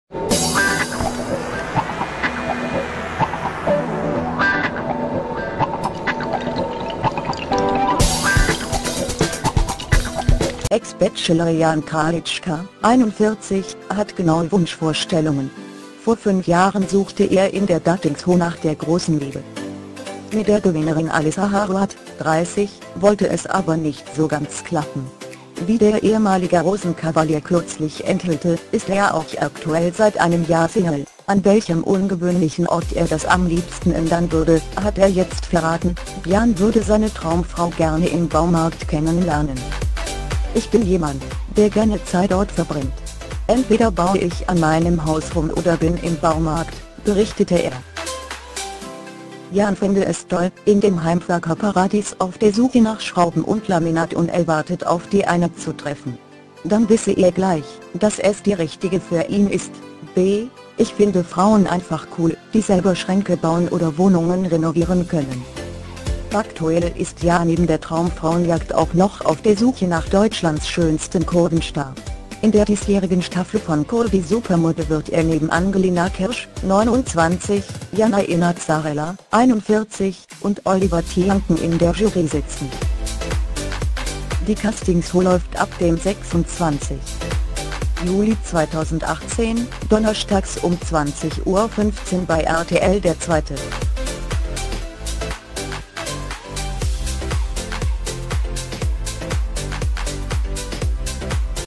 JAN KRALITSCHKA: ER WILL SEINE IM BAUMARKT TREFFEN
Ex-Bachelor Jan Kralitschka 41 hat genaue Wunschvorstellungen! Vor fünf Jahren suchte er in der Datingshow nach der großen Liebe.
 Mit der Gewinnerin Alissa Harouat 30 wollte es aber nicht so ganz klappen. Wie der ehemalige Rosenkavalier kürzlich enthüllte, ist er auch aktuell seit einem Jahr single – an welchem ungewöhnlichen Ort er das am Liebsten ändern würde, hat er jetzt verraten: bJan würde seine Traumfrau gerne im Baumarkt kennenlernen!.